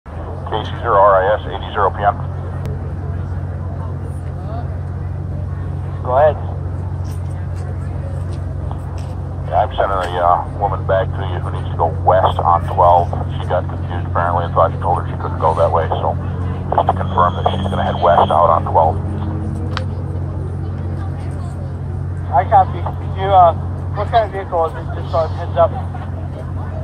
Arlidazzle 2016, Arlington MN
My Mustang I rented for the week's stay in MN.